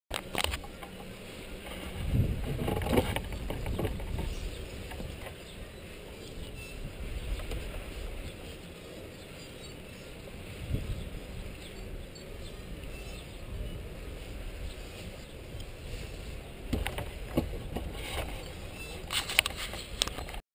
Testvideo Hero GoPro auf SY Petroula
Neue Perspektive vom Heckmast aus nach vorne